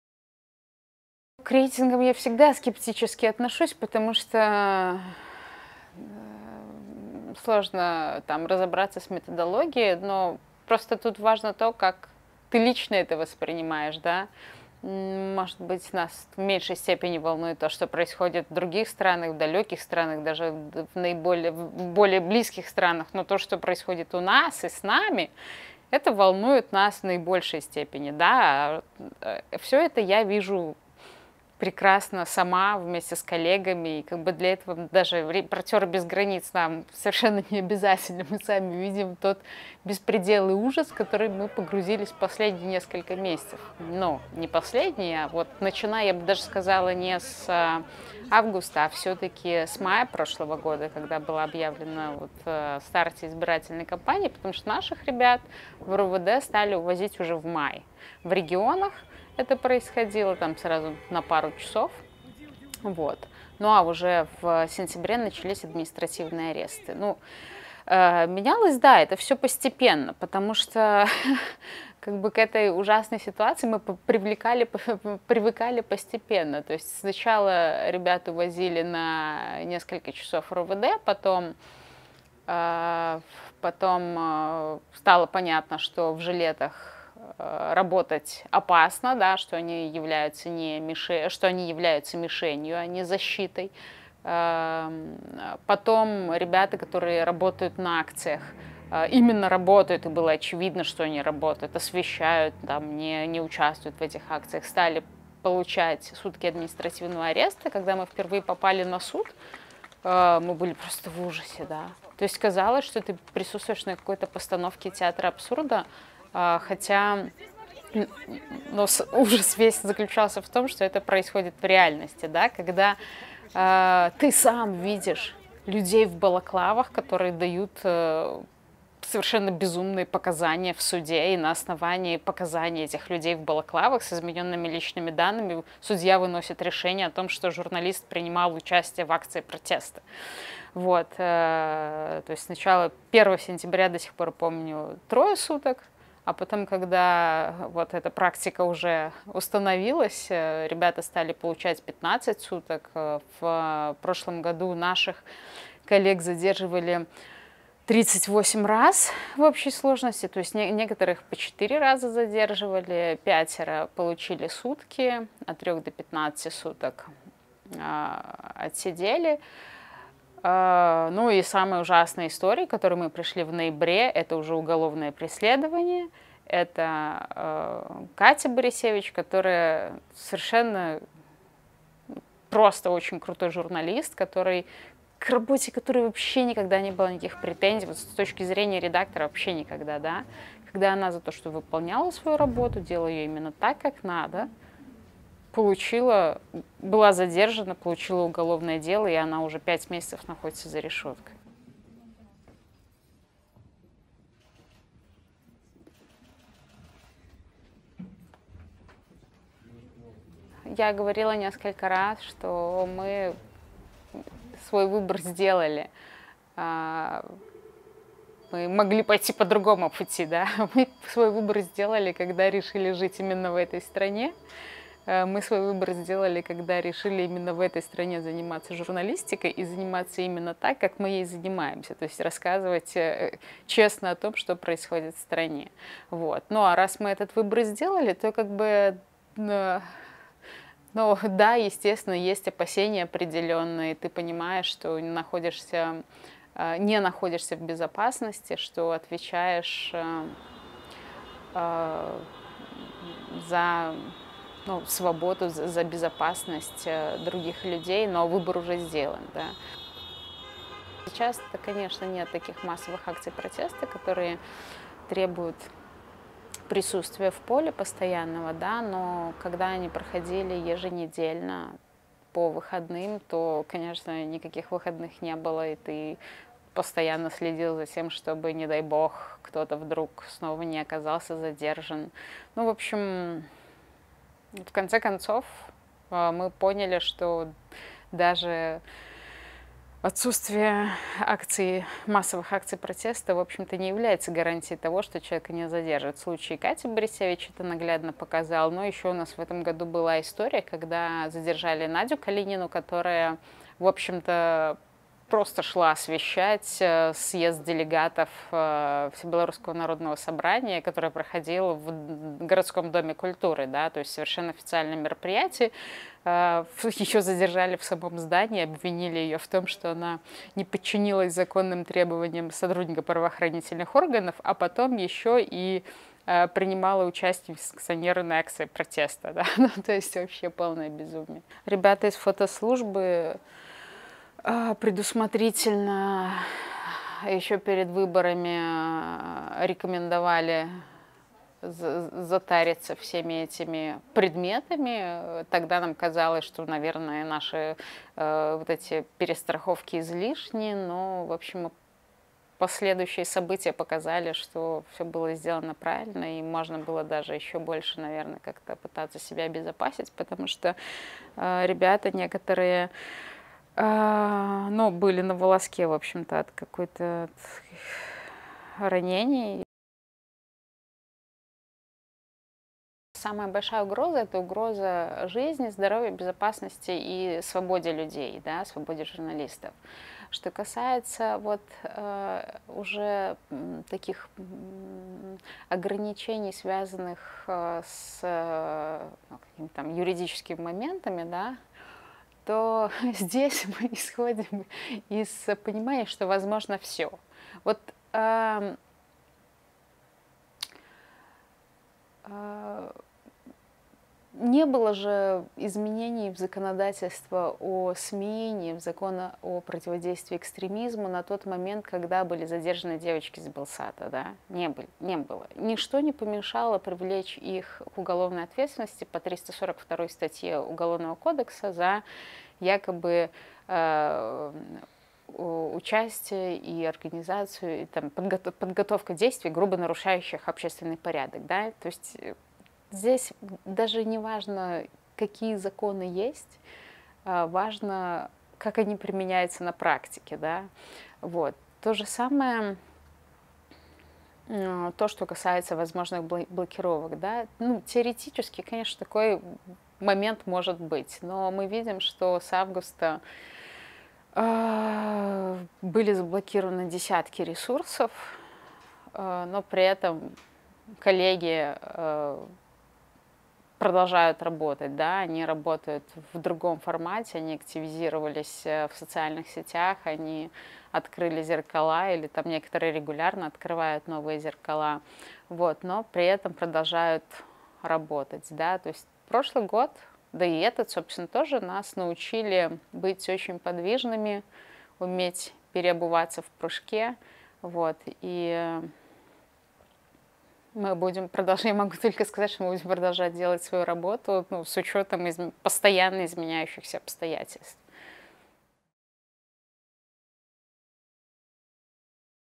"Прошлый год научил нас переобуваться в прыжке" - главный редактор TUT.by
Еще до своего задержания и блокировки TUT.by главный редактор портала Марина Золотова в интервью DW рассказала о том, как изменилась работа журналиста в Беларуси после 9 августа 2020 года. Мы планировали использовать части этого интервью в специальном проекте DW 23.34. Но после того, как @MAPAvideo_   подвергся преследованию в Беларуси, мы решили опубликовать рассказ о том, в каких условиях портал работал с мая прошлого года.